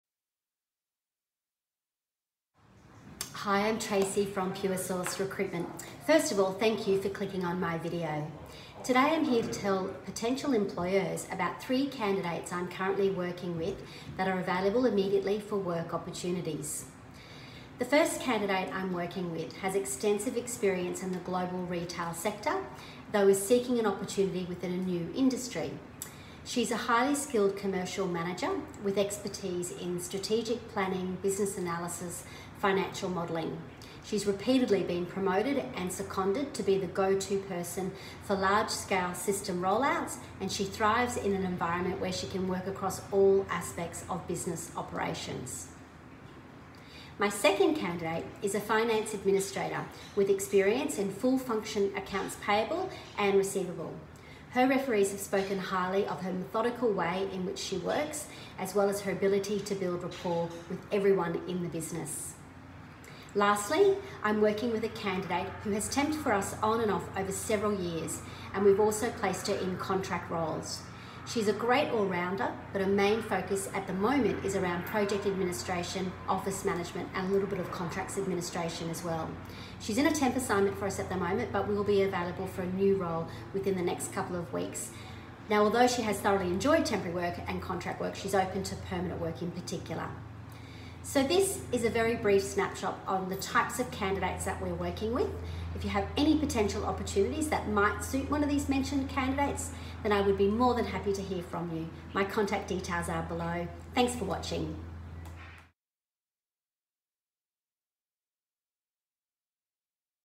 A snapshot of candidates I’m currently working with
I'm working with some exceptional candidates at the moment, here is a quick summary of just a few.